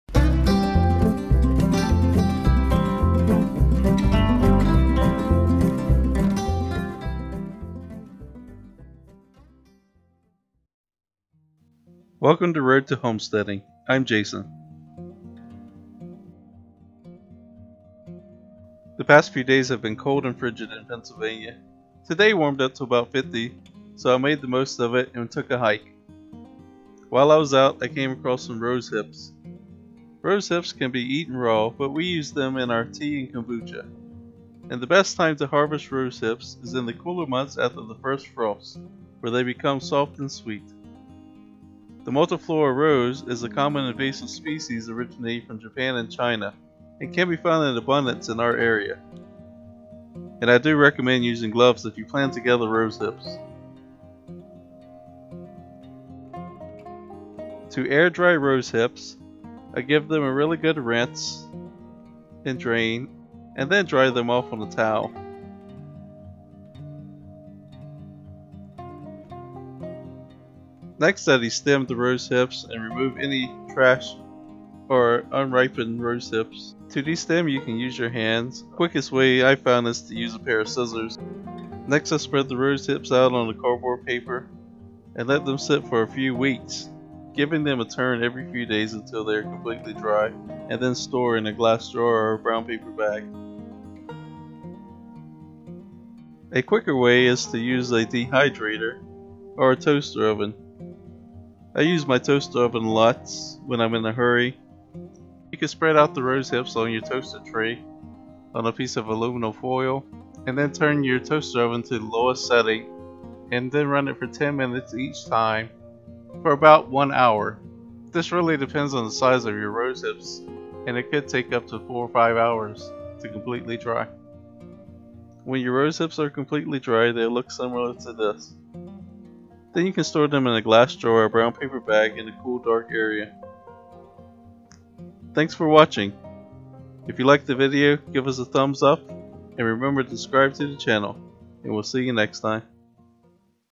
Gathering and Drying Rose Hips
The past few days have been cold and frigid in PA, today warmed up to 50 so I made the most of it and took a hike. While I was out I came across some rose hips. Rose hips can be eaten raw, but we dry them to use in our teas and Kombucha. The best time to harvest rose hips is in the cooler months after the first frost where they become soft and sweet.

The Multi-flora Rose is a common invasive species originating from to Japan and China, and can be found abundantly in our area. The rose hips of the Multi-flora Rose are small, unlike some cultivated varieties. I do recommend using gloves if you plan to gather rose hips.

We Use:
Canon t1i (need an upgrade)
GoPro HERO4 BLACK
GoPro HERO5 Session
Blackmagic Design DaVinci Resolve Studio